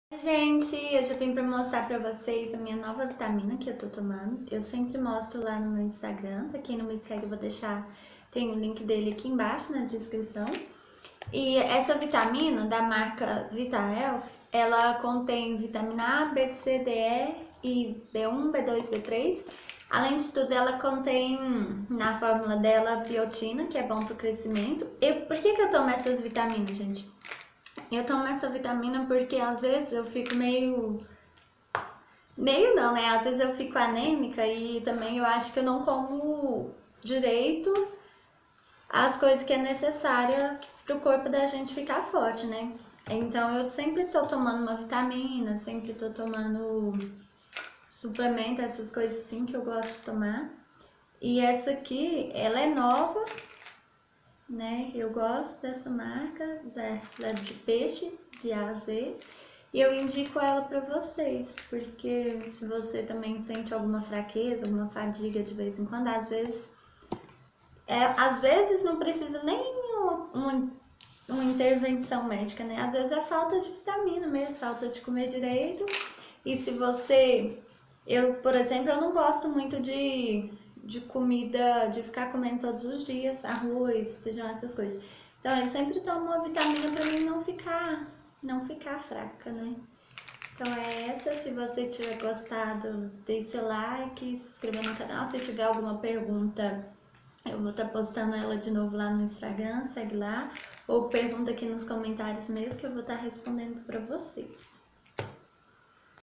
Vitahealth de A - Z ômega 💊❤ (Jessy Duarte)
Esse é o polivitaminico que tenho tomado esses dias.. 🎤🎶